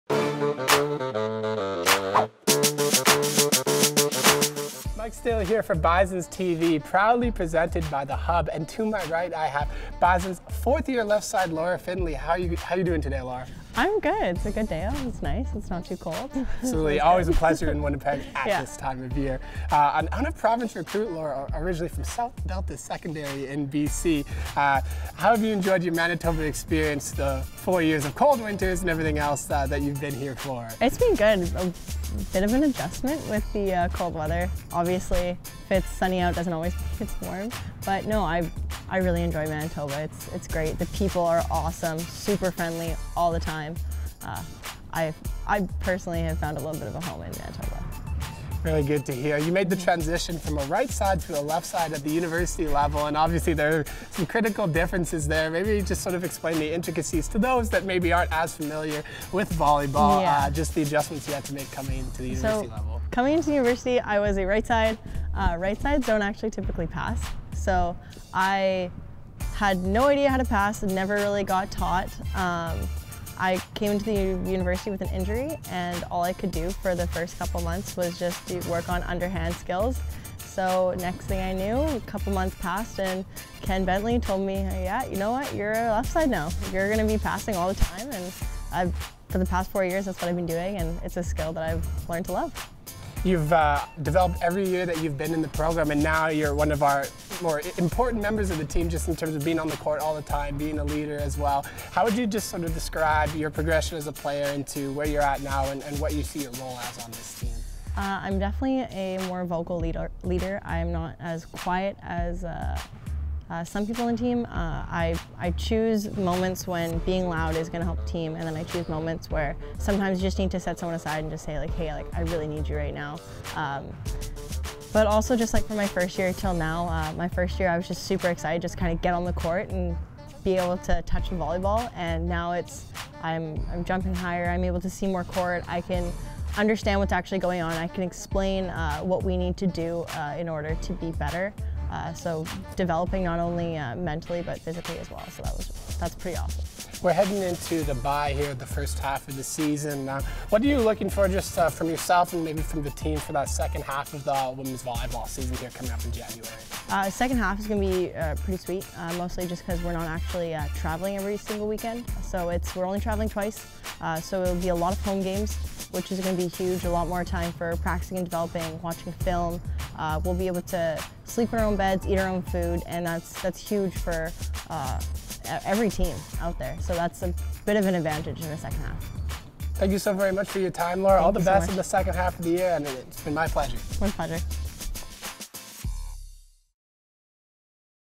Laura Findlay, Bison Women's Volleyball | BisonsTV
In this week's BisonsTV, we talk to fourth-year left-side Laura Findlay about her experience in living in Winnipeg, switching positions, and what to expect in the second half of the season.